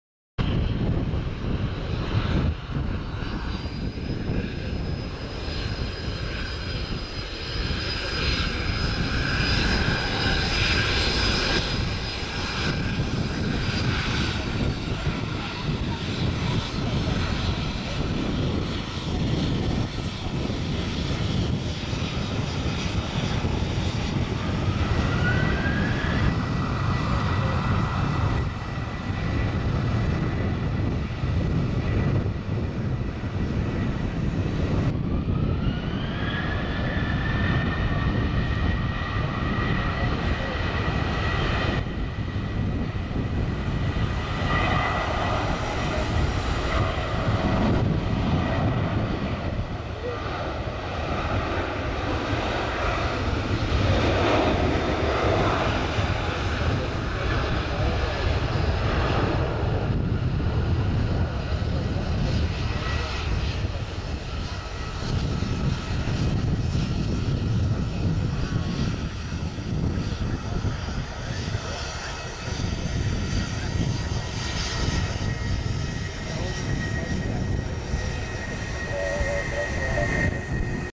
Qantas Boeing 737-838(WL) | Brisbane Airport- YBBN
Welcome back to another video. This is another aircraft spotting video at Brisbane Airport. This is one of the many will be uploaded in the upcoming weeks.

Flight Number- ?
Date- 27th January 2014
Airline- Qantas
Aircraft- Boeing 737-838(WL)
Rego- VH-VZO
Time- 11:12am EST